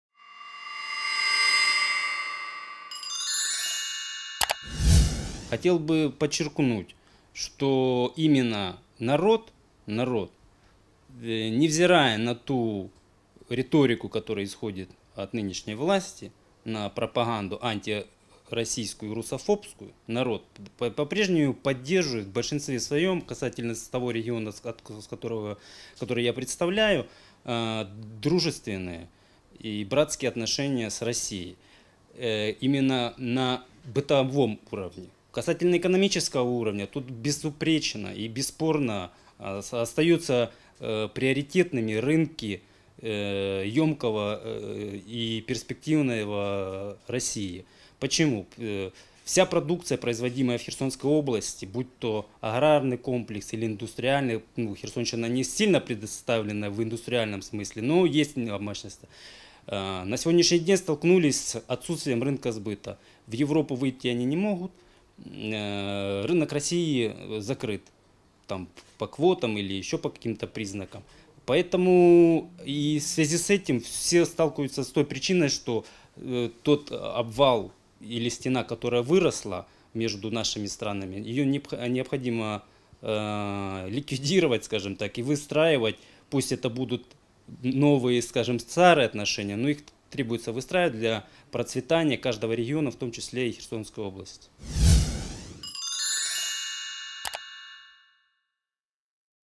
Украина должна дружить с Россией
Андрей Клочко, руководитель территориального совета Движения "Украинский выбор — Право народа" в Херсонской области, рассказывает, как он видит нынешние отношения Украины с соседними государствами, в частности с Россией.

Журналисты: Татьяна Дидан, Ирина Опря
Оператор: Александр Детыненко